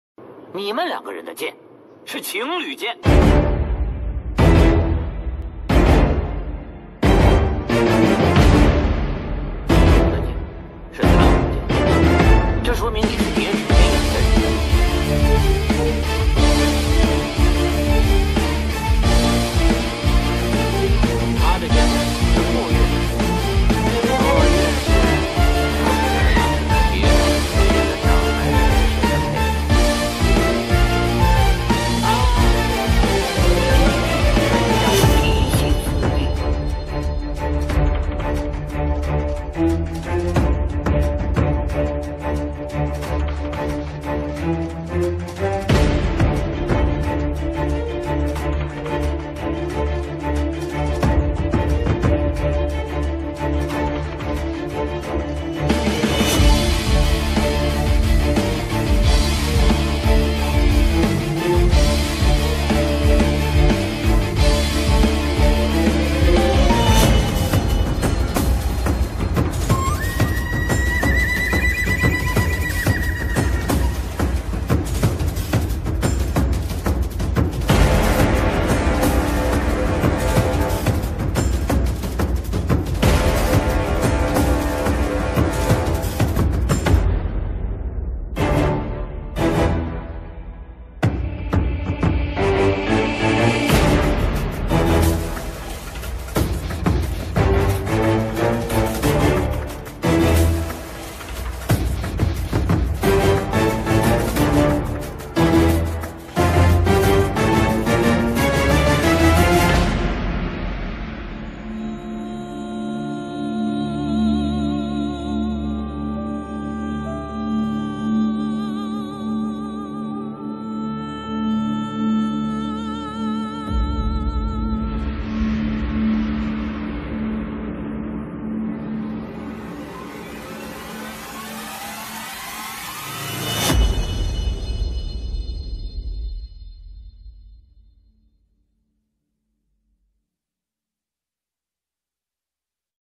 LOVERS SWORDS|POYUE JIAN - CANHONG JIAN - SWORDS OF THE HEIR AND SPY OF THE EYES OF GOD |Elma Bayug
The two swords called LOVERS SWORDS, these are the ...
⭐CANHONG JIAN - this is the sword that Master Yuwen Yue gave to Xing'er. CANHONG Jian is the sword of the Spy of the Eyes of God. That means Xing'er is now a full-fledged spy of the Yuwen Household.
🌙POYUE JIAN- this belongs to the head of the Eyes of God, which means Master Yuwen Yue is the inheritor of the Yuwen Family.

Fb: Elma Vinasoy Bayug
Ig : Elma Vinasoy Bayug